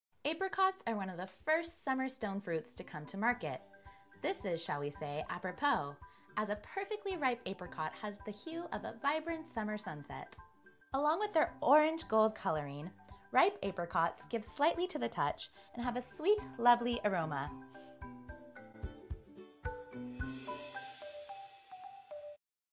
Ode to the Apricot | The FruitGuys
Apricots are one of the first summer stone fruits to come to market. This is, shall we say, apropos, as a perfectly ripe apricot has the hue of a vibrant summer sunset. Along with their orange-gold coloring, ripe apricots give slightly to the touch and have a sweet, lovely aroma.